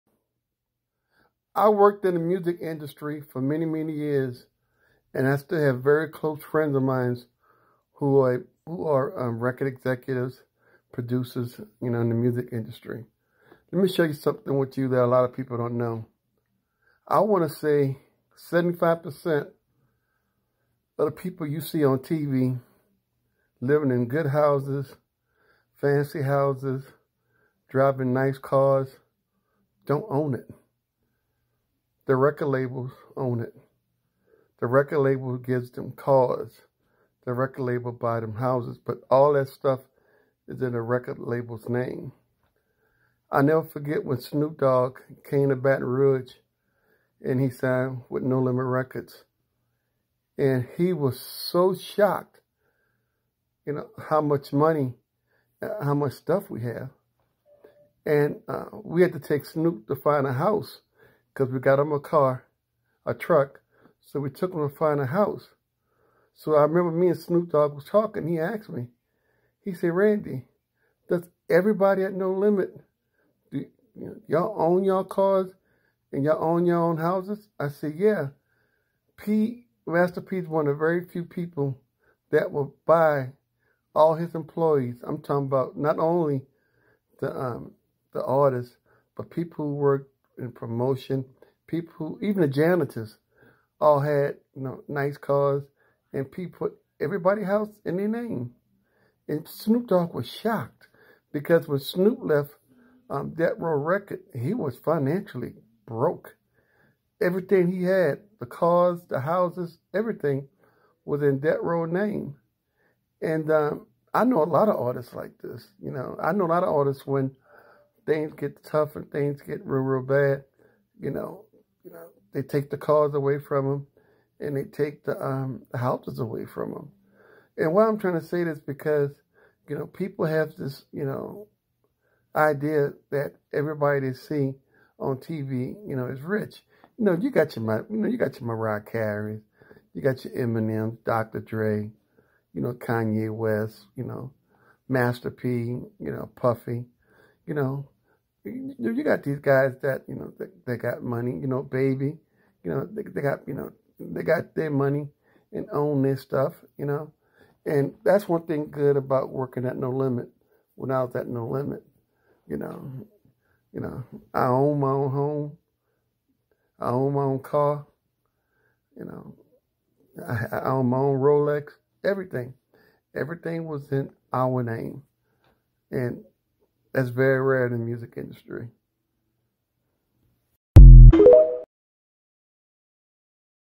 The truth about the music industry.
I work for 15 years in the music industry. Here is my story about the music industry.